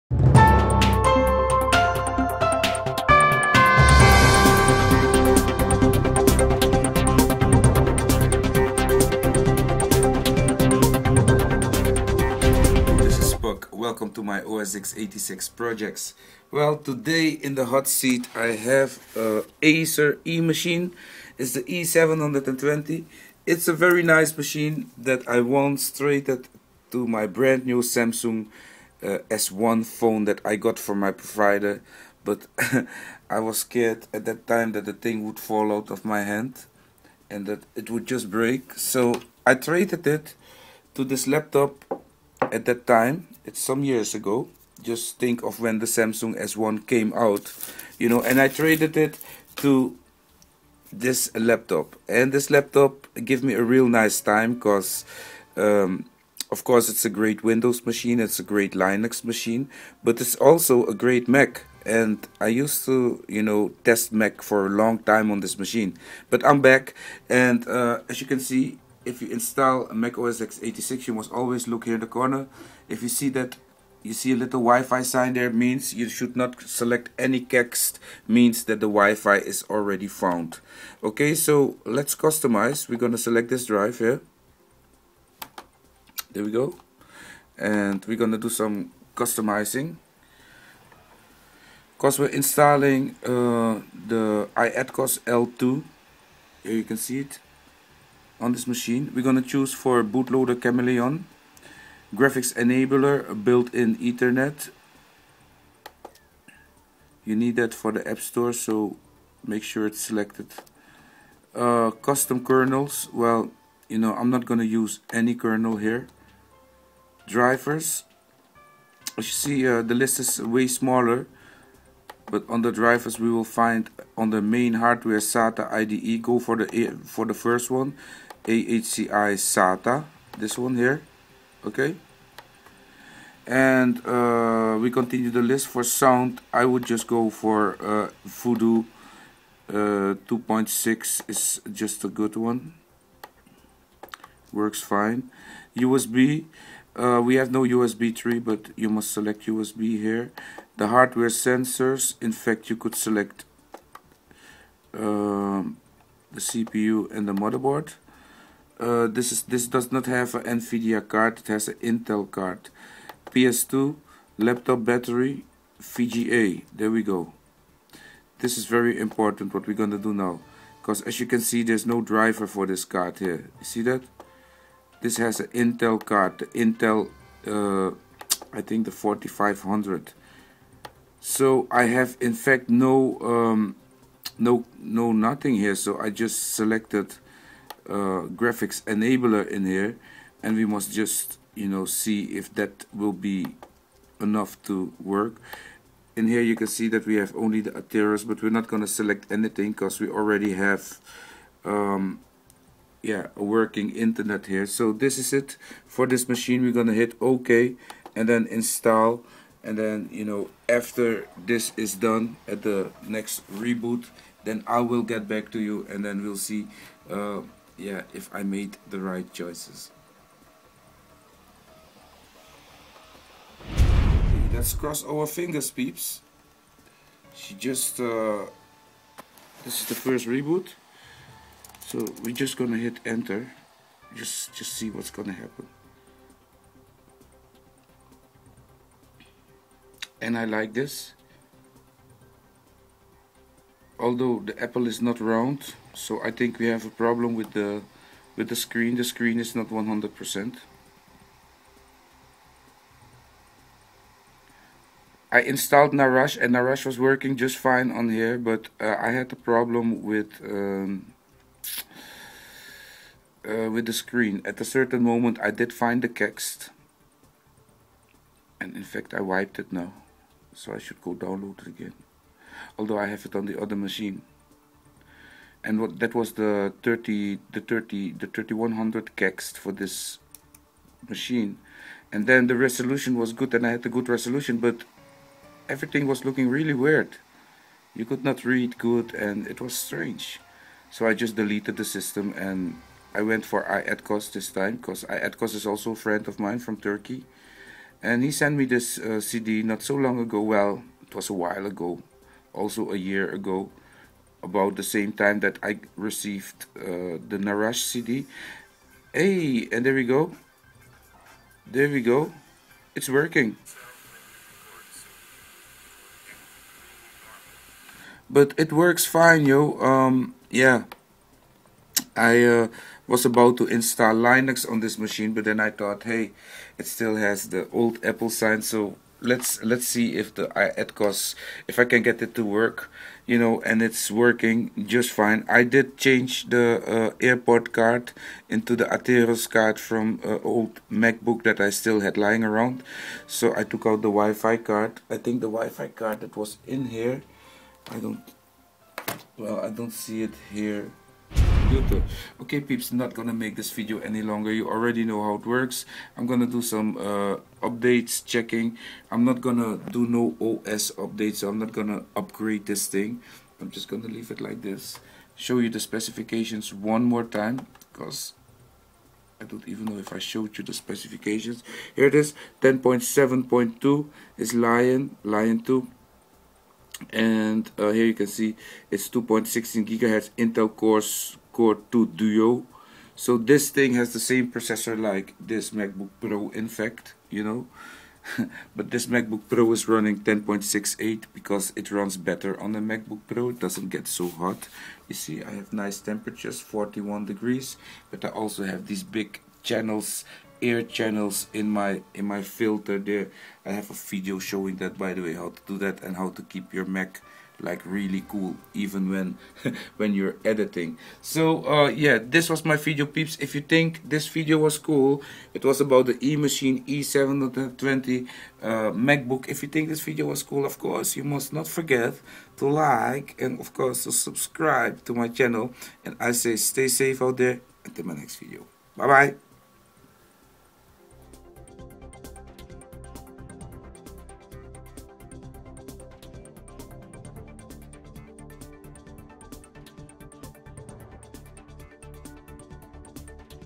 HOW TO TEST INSTALL MAC OS X86 ON THE ACER EMACHINE e720 FULL INSTALL + TIPS - 2015
MAC OSX88  project with the Acer Emachine e720
a great laptop that can be a Linux Windows or MAC - Amazing machine.

To make Airport always function , I removed the broadcom that worked until 10.6.3 iATKOS S3 - for a original Airport card Atheros from a old Macbook pro ..

The settings will be clear and so the tips you will get..

This can also be done on another Laptop if you know the Hardware..It's all about the hardware..so study it.

By Puck Darlington
Test Project 2015